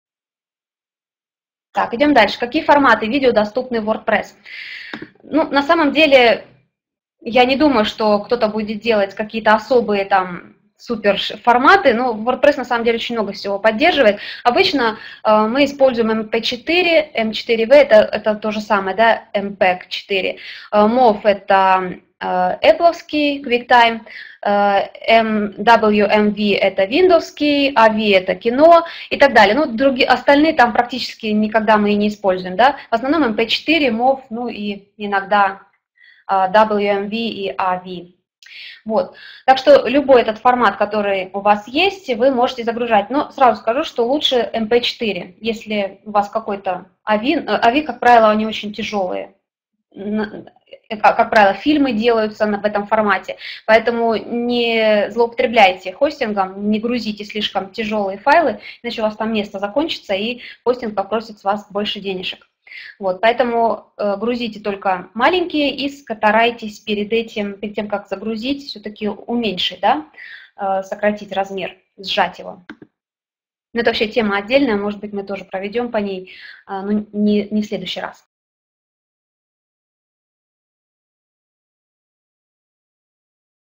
Урок 27-3. Видео с хостинга. Какие форматы видео допустимы в WordPress.
Видео с хостинга. Какие форматы видео допустимы в WordPress.
Это видео курса "Администратор блога на WordPress".